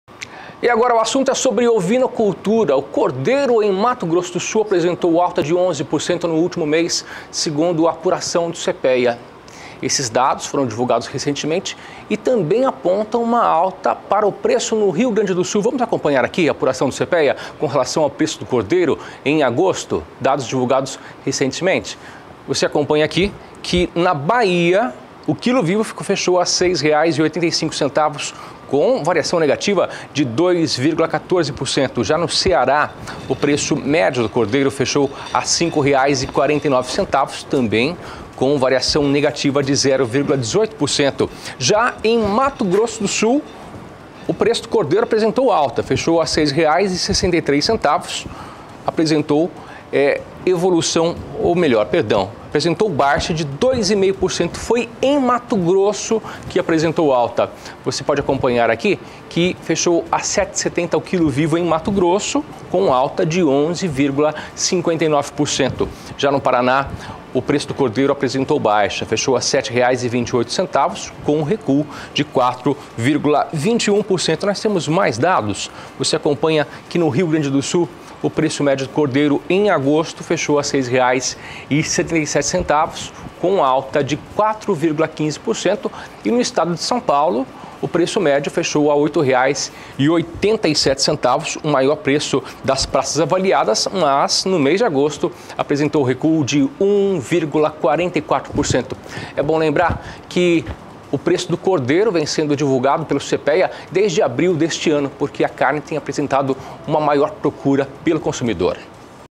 Carne de cordeiro em Mato Grosso apresentou alta de 11% no último mês - 22/09/2017
Na apuração do Cepea, a carne do animal também teve valorização no Rio Grande do Sul. Acompanhe o preço do cordeiro nas principais praças de produção do país.